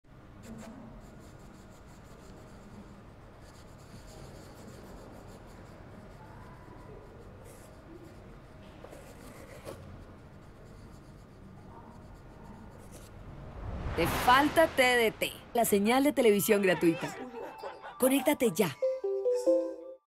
Saca lo mejor de tu televisor con la señal TDT
¡Que no te falte TDT! Sintoniza la señal HD de la TDT para aprovechar al máximo la imagen que pude ofrecer tu televisor 📺.

Línea de atención al usuario: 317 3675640 (Lunes a viernes de 8 am a 6 pm)